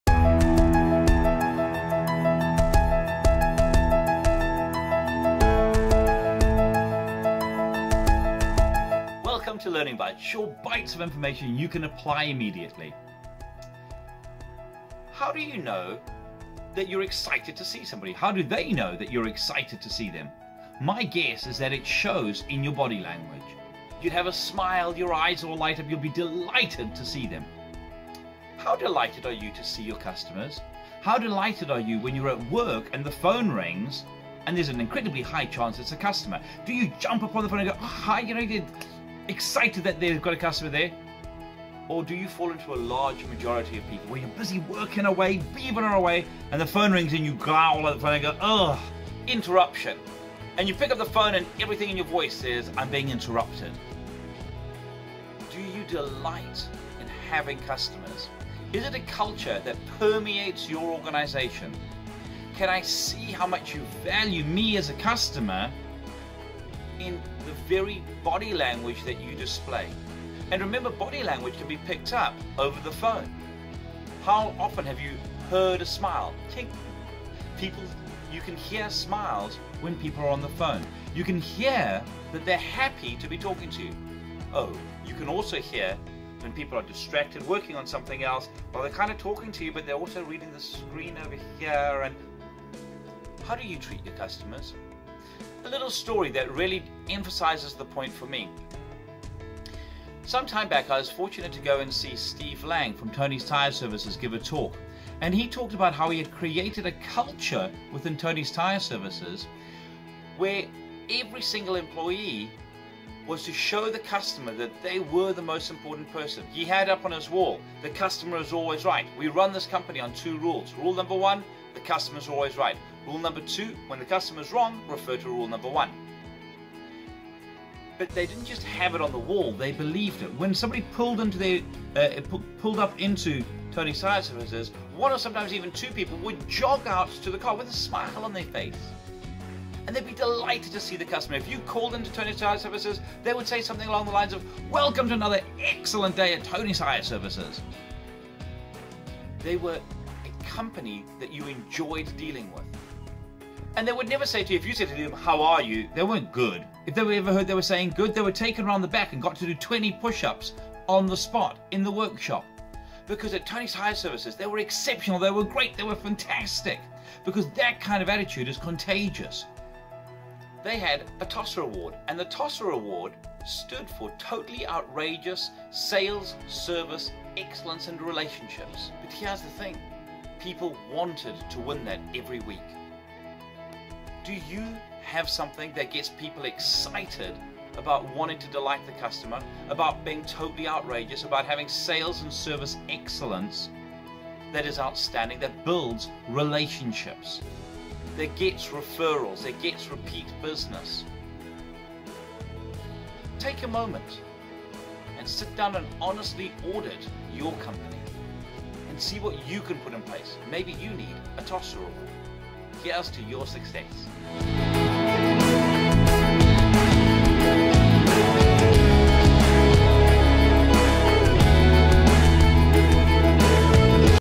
TRLB #44 Customer Service - How Do You Treat Your Customers
Does your business have the hallmarks of great customer service?  From body language to tone of voice, are your staff trained to make your customers feel exceptional?  With real life examples and target questions, explore with Mike how to give your customers the best.